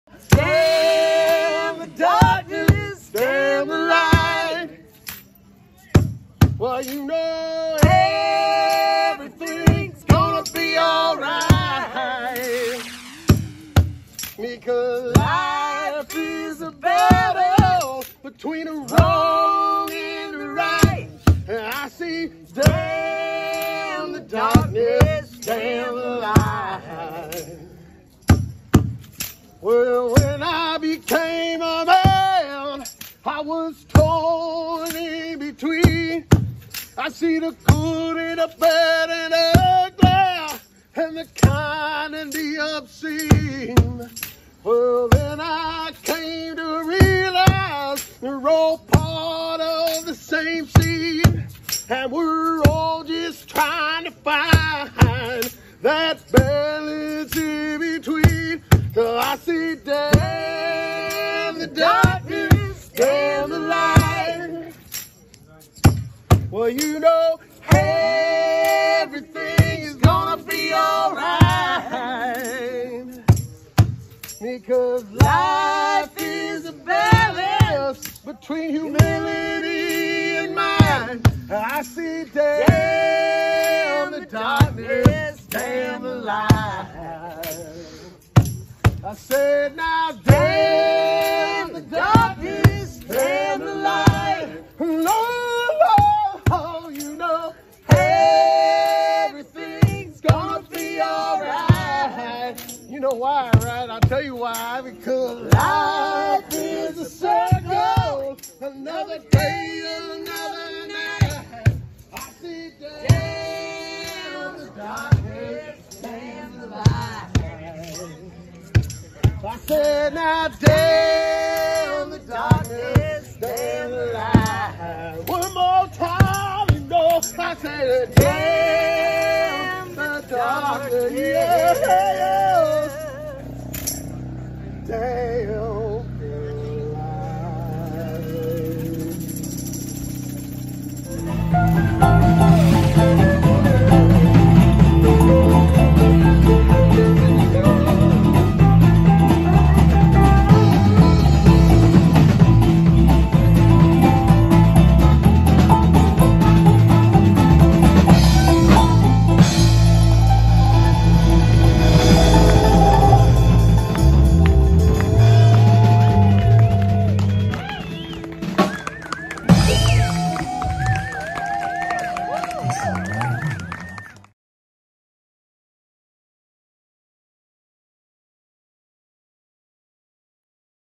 Tamarack Camp: A night in October 2020 with Beaver Xing and Smokin' Dobroleles
Photo gallery at LocalSpins.com: Beaver Xing and The Smokin' Dobroleles performing at Tamarack Camp on Oct. 10, 2020.